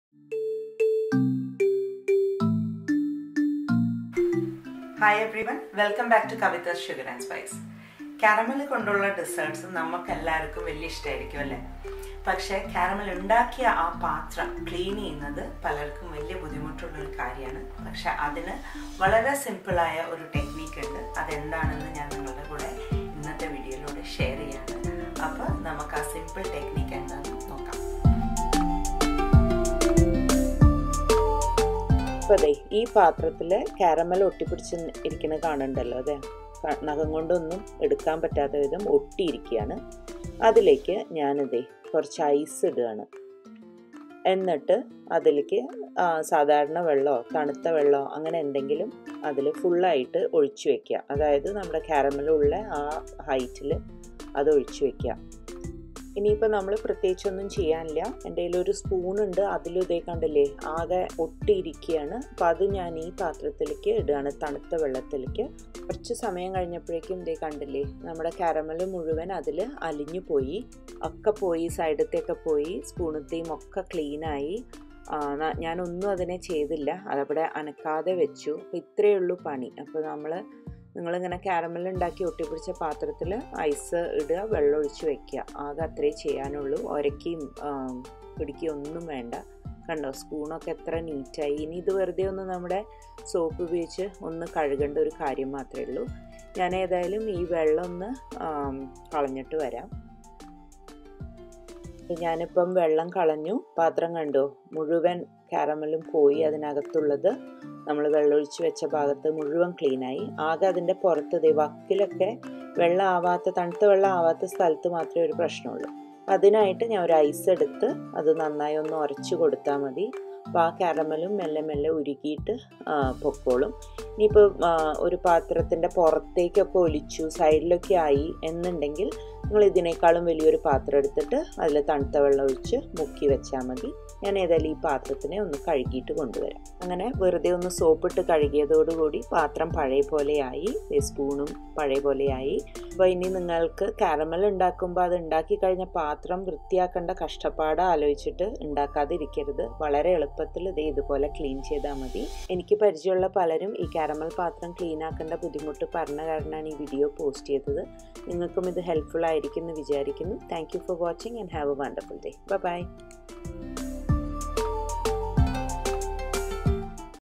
Easily Clean a Caramel Vessel || Easy Kitchen Tips in Malayalam
Easily clean a vessel you used to make caramel. No scrubbing, no sticky mess.  Don't let the thought of the cleaning stop you from making your favourite dessert.

Music courtesy:
• Follow djshahmoney: